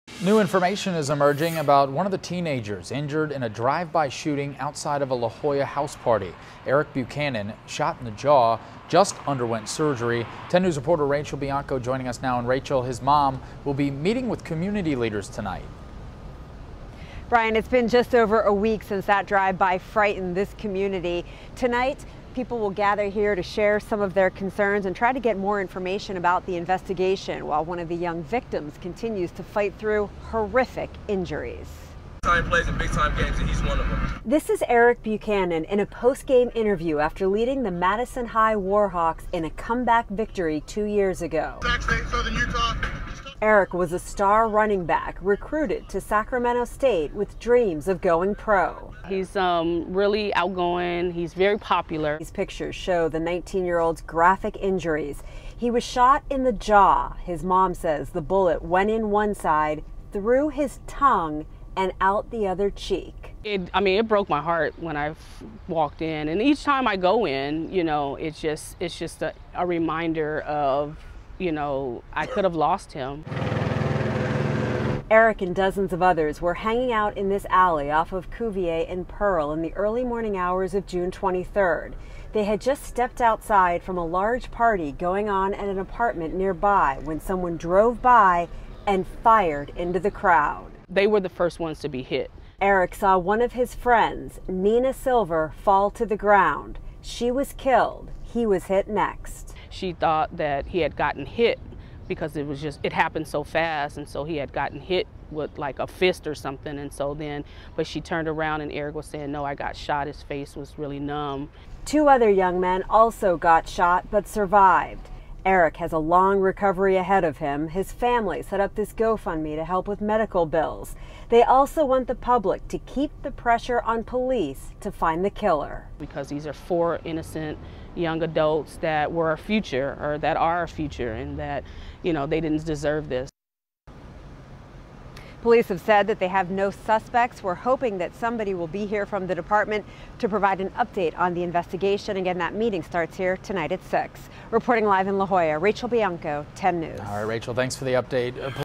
Teen injured in drive-by undergoes surgery
Erick Buchanan was one of three people shot in La Jolla.